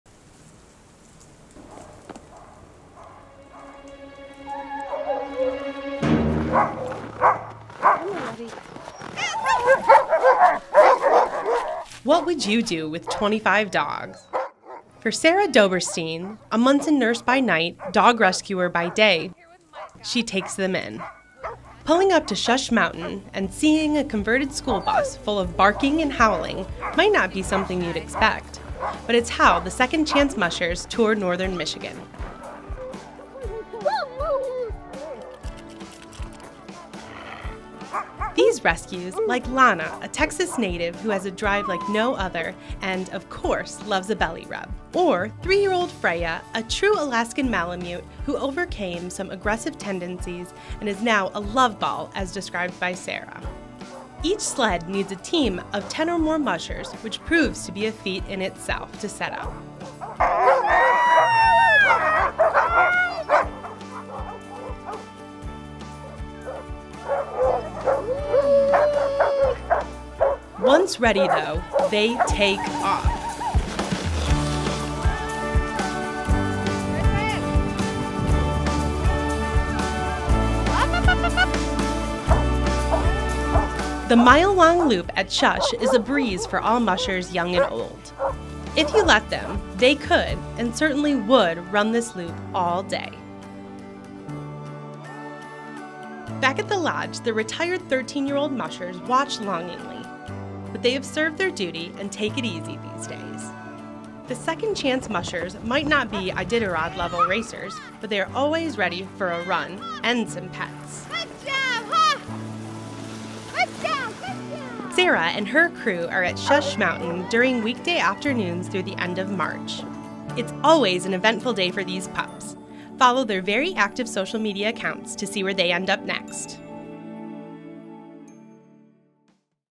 Go Dog Sledding in Northern Michigan with Second Chance Mushers
You’ve heard of the crazy cat lady? Meet her remarkably logical foil, Sarah Dobbrastine—Munson nurse by night; indefatigable mom of 25 huskies by day. Here’s how she’s saved these pups, has given them a second chance at life and turned her passion into a Northern Michigan nonprofit